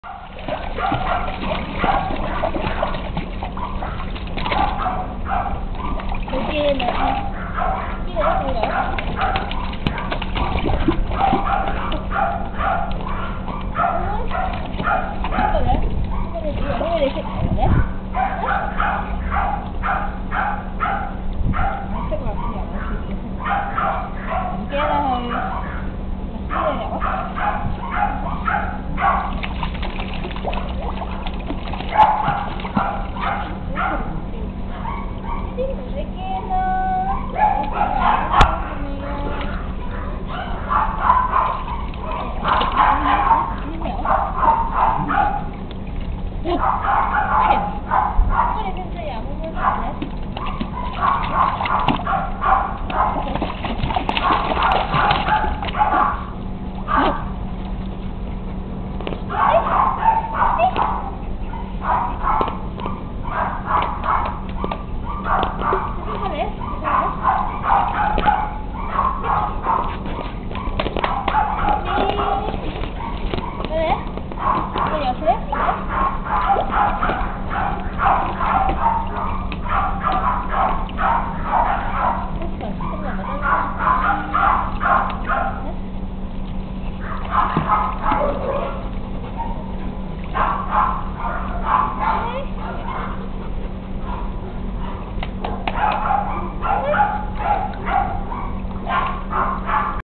Westie Babies Swimming Lesson I
Rowan Family...three little babies started their swimming lesson last Saturday. Let's share ..they are having fun. Cheers!!